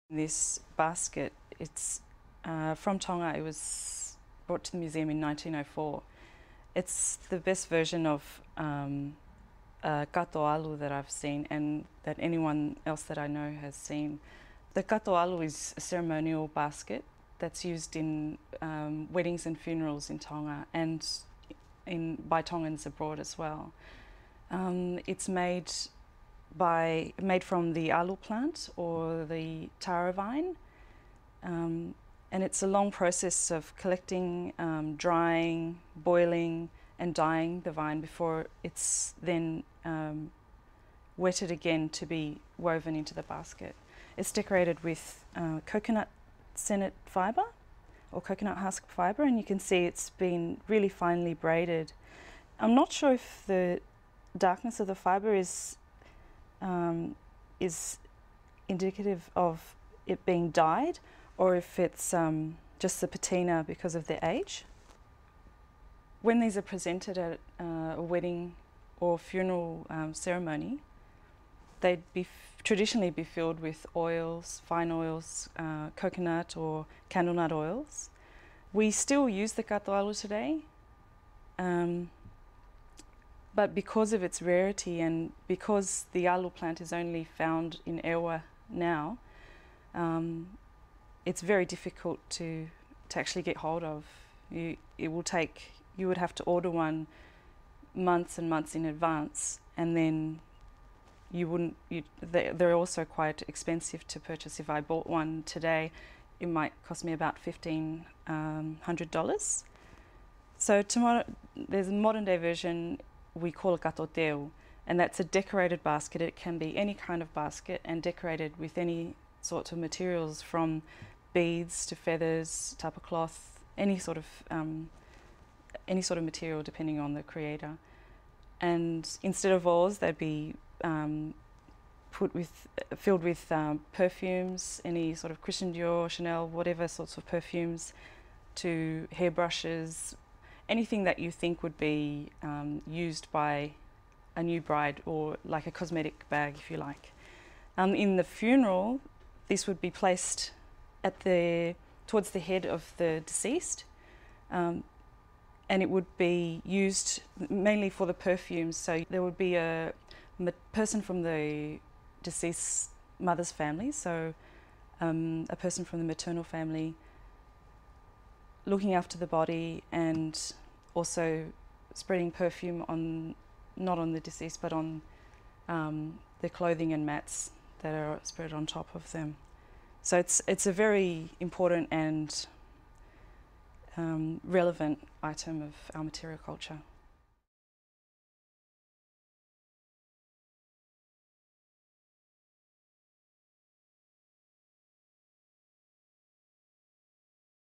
Kato Alu Basket
Eseta Aholelei tells us about a Tongan Kato Alu Basket and its use in ritual.

Rights: © Australian Museum and Eseta Aholelei
Producer: Finton Mahony
Presenter: Eseta Aholelei

Transcript

This basket, it's from Tonga. It was brought to the museum in 1904. It's the best version of katu alo that I've seen, that anyone else that I know has seen. The katu alo is a ceremonial basket that's used in weddings and funerals in Tonga, and by Tongans abroad as well.

It's made by - made from the alo plant, or the taro vine. And it's a long process of collecting, drying, boiling and dyeing the vine before it's then wetted again to be woven into the basket. It's decorated with coconut sennet fibre, or coconut husk fibre, and you can see it's been really finely braided.I'm not sure if the darkness of the fibre is indicative of it being dyed, or if it's just the patina because of the age. When these are presented at a wedding or a funeral ceremony they'd traditionally be filled with oils, fine oils, coconut or candlenut oils.

We still use the katu alo today, but because of its rarity and because the alo plant is only found in Eua it's very difficult to actually get hold of. It would take, you would have to order one months and months in advance, and then you wouldn't - they're also quite expensive to purchase. If I bought one today it might cost me about $1500.

So there's a modern-day version we call a katu tao and that's a decorated basket, it can be any kind of basket, and decorated with any sorts of materials, from beads to feathers, tapa cloth, any sort of - any sort of material, depending on the creator. And instead of oils they'd be put with - filled with perfumes, any sort of Christian Dior, Chanel, whatever sorts of perfumes, to hairbrushes - anything that you think would be used by a new bride - or like a cosmetic bag if you like.

In the funeral, this would be placed at the - towards the head of the deceased, and it would be used mainly for the perfumes. So there would be a person from the deceased's mother's family, so a person from the maternal family, looking after the body and also spreading perfume on - not on the deceased but on the clothing and mats that are spread on top of them.

So it's a very important and relevant item of material culture.